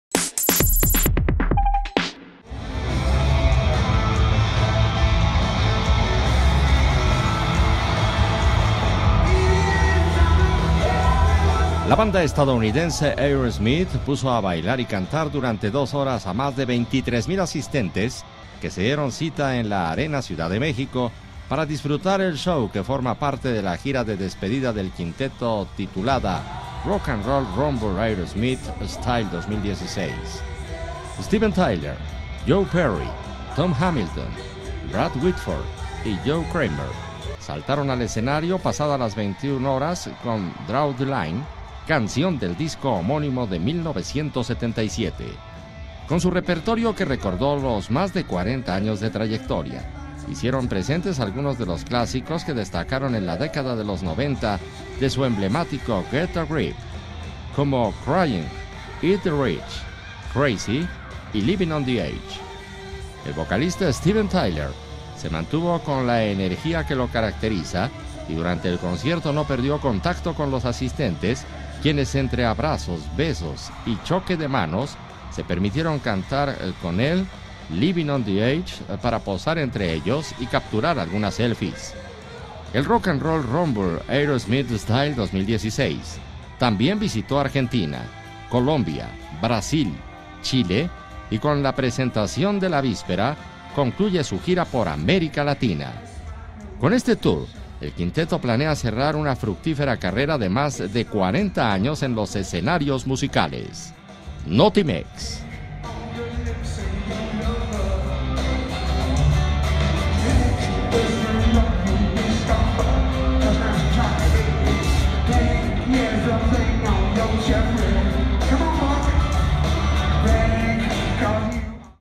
Aerosmith pone a cantar y bailar a fans con "show" colmado de clásicos
Con este "Tour" el quinteto planea cerrar una fructífera carrera de más de cuarenta años en los escenarios musicales.